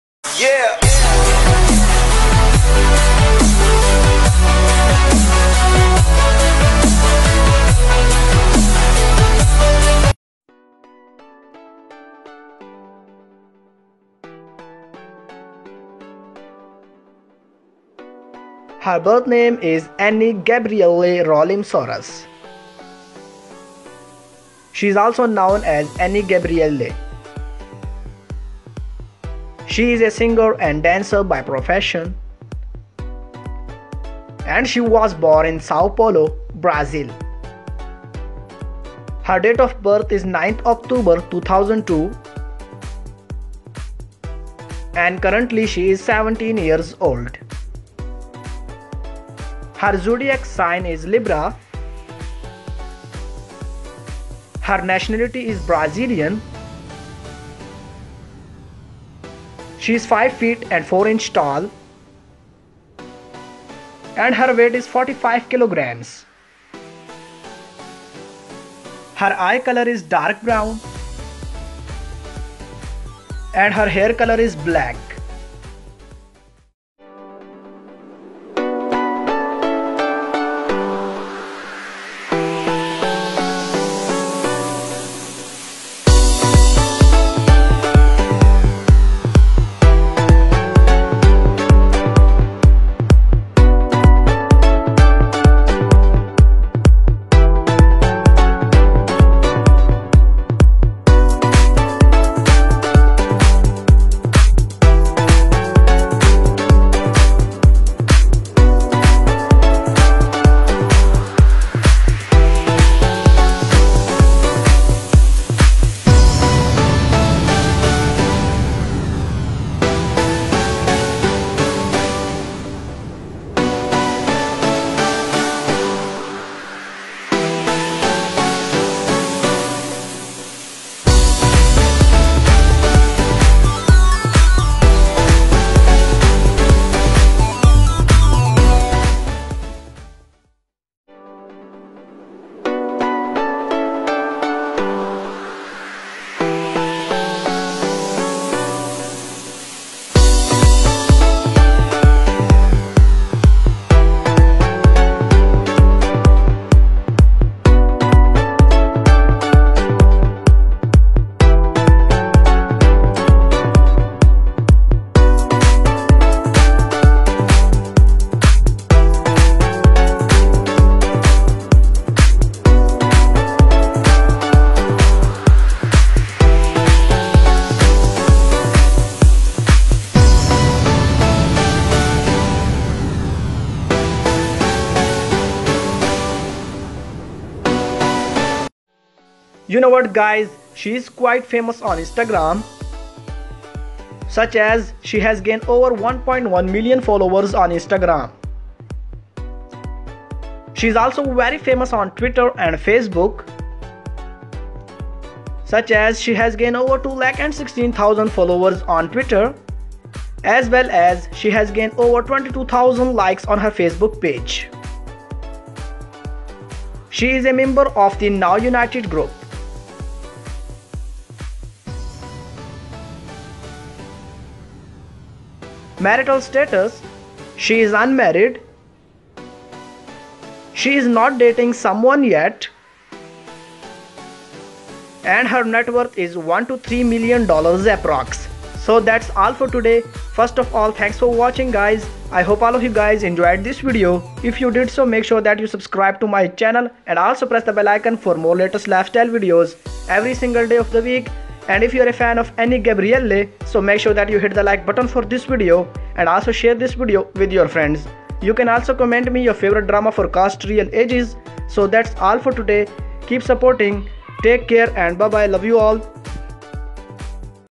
Any Gabrielly (Now United) Lifestyle | Family | Facts | Net Worth | Biography | FK creation
THE MOST POPULAR AND VERY TALENTED MEMBER OF THE NOW UNITED ANY GABRIELLY LIFESTYLE AND BIOGRAPHY VIDEO BY FK CREATION 2019